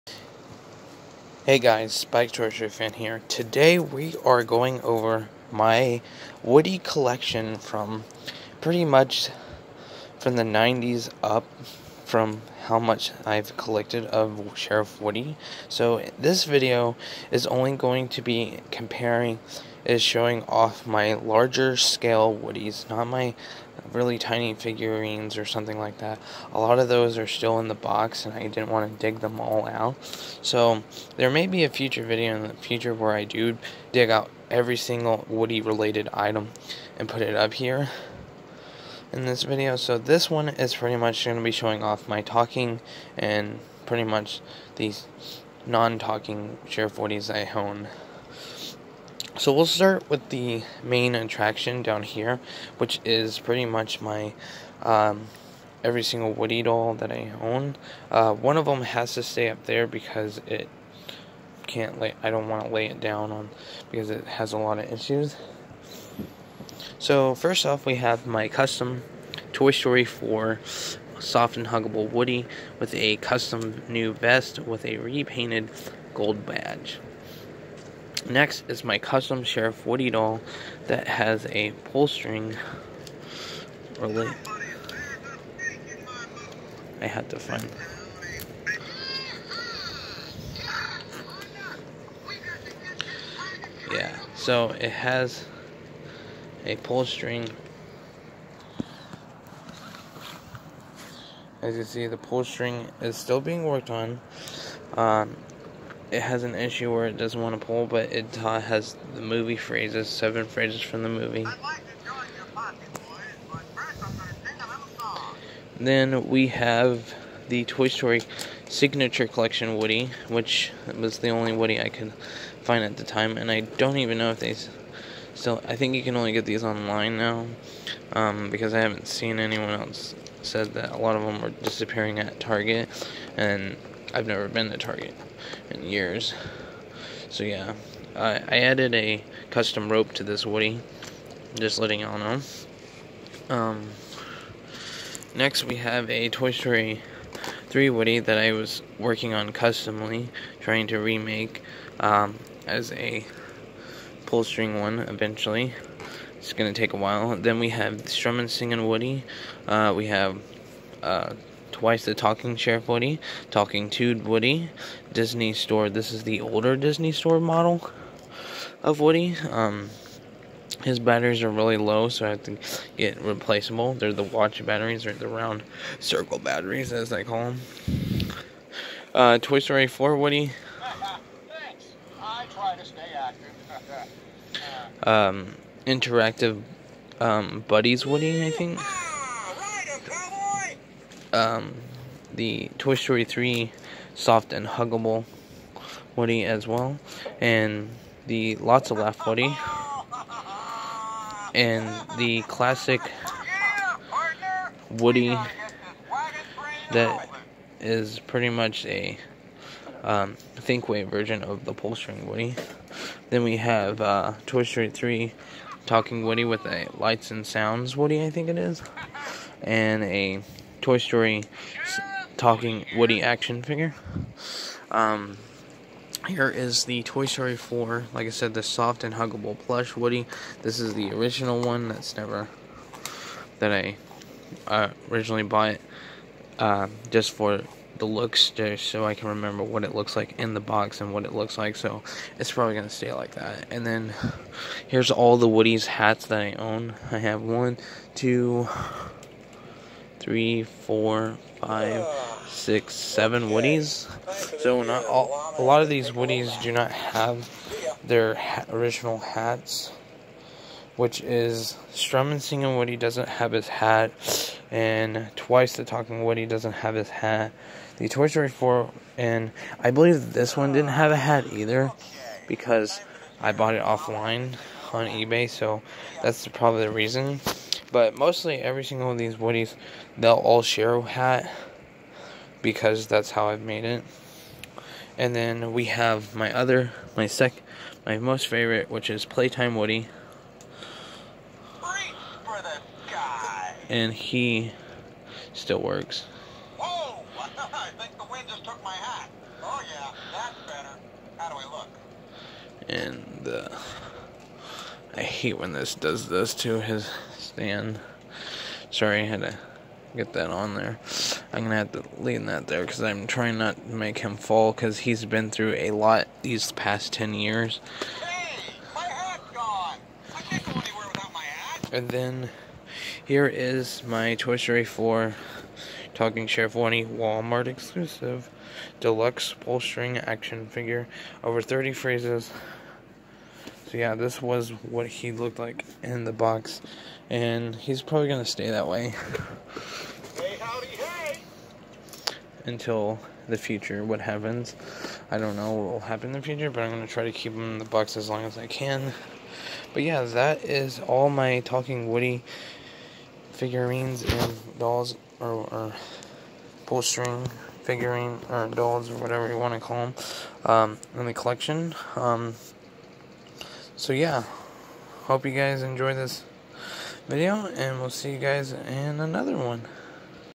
My Sheriff Woody collection
So this is my collection of all my talking and non talking woodys I had for years.